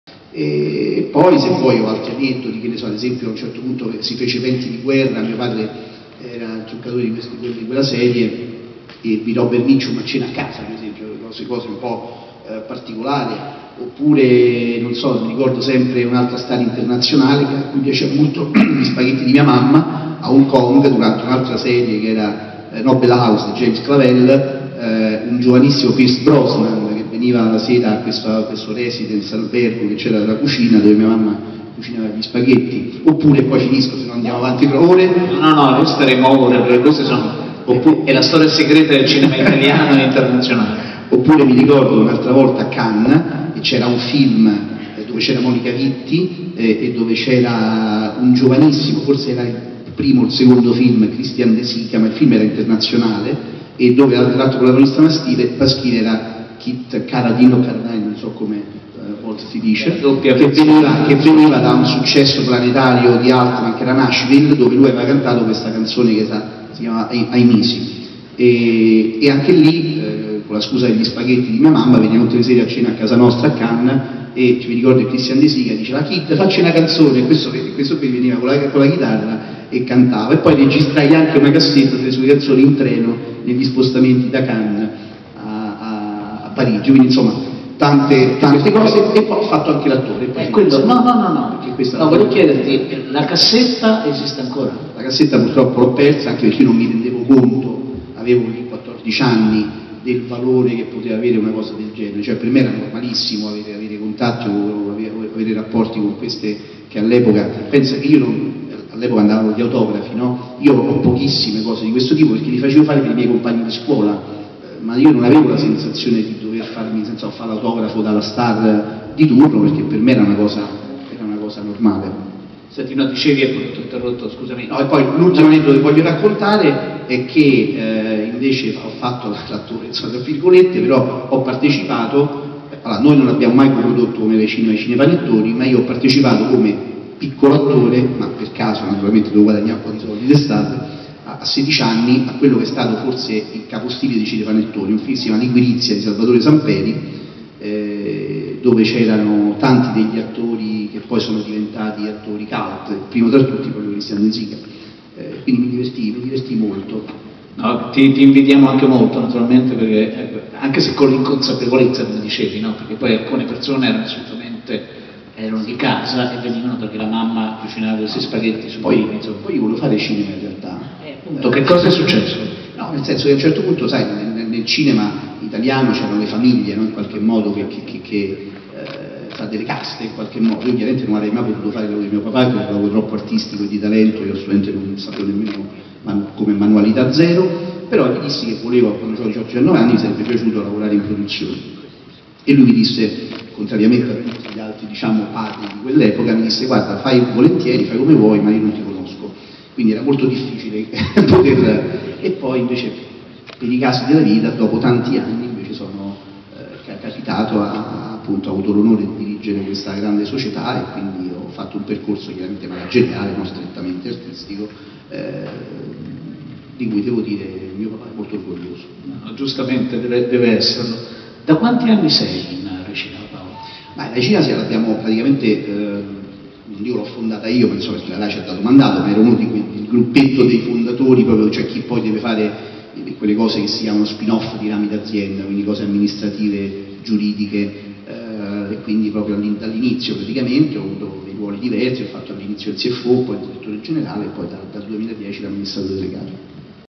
Paolo Del Brocco al BIF&ST 2019: i ricordi di Robert Mitchum e Pierce Brosnan
- Per Paolo Del Brocco, amministratore delegato di Rai Cinema al BIF&ST 2019 per una masterclass a lui dedicata, era normale avere a che fare con le star del momento. Durante le riprese della miniserie Venti di guerra l'attore Robert Mitchum è stato tante volte a casa sua. Pierce Brosnan amava gli spaghetti di sua madre, è andato a Cannes con Monica Vitti e Christian De Sica ed ha anche fatto la comparsa come piccolo attore in Liquirizia di Salvatore Samperi.